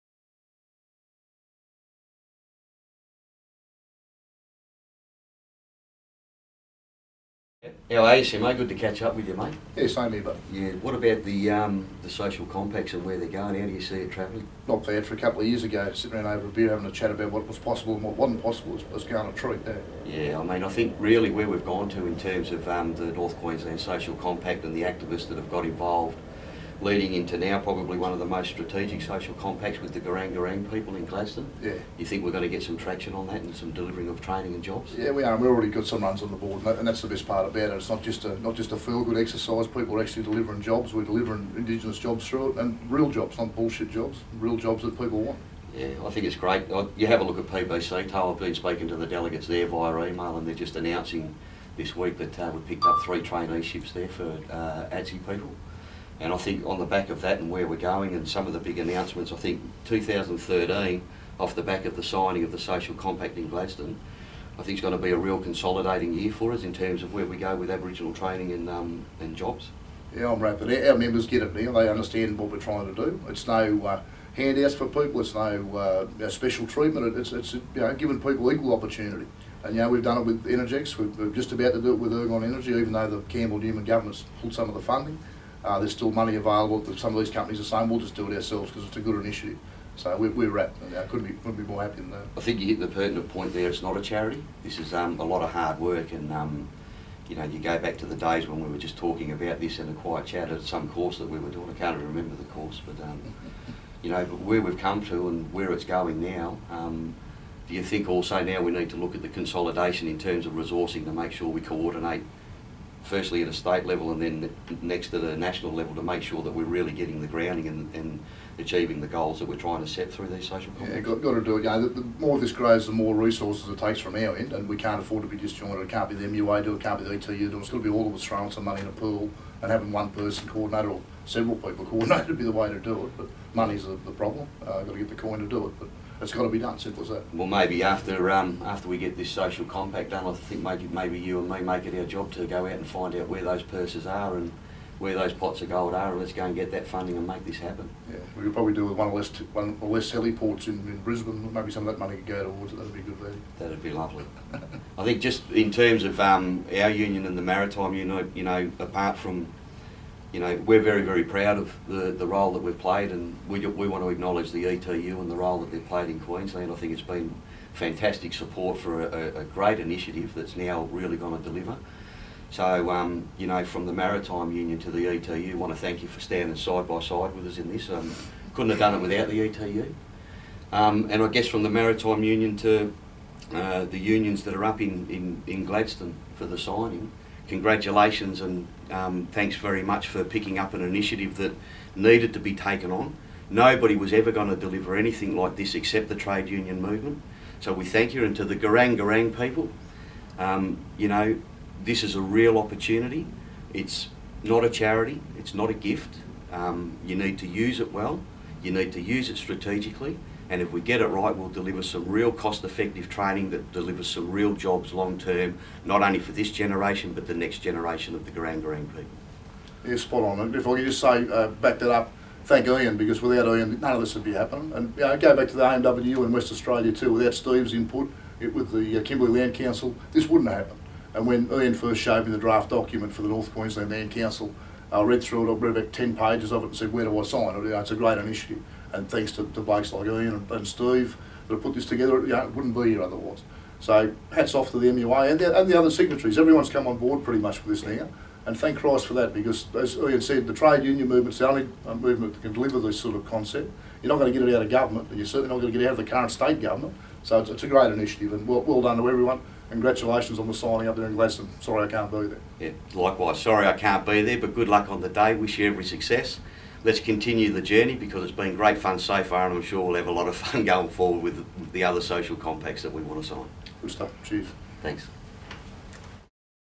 Social Compact Agreement - Gureng Gureng Peoples
Ian Bray (MUA) and Peter Simpson (ETU) discuss the social compact heads of agreement between the Gureng Gureng Peoples and the Queensland Council of unions and Signatory unions.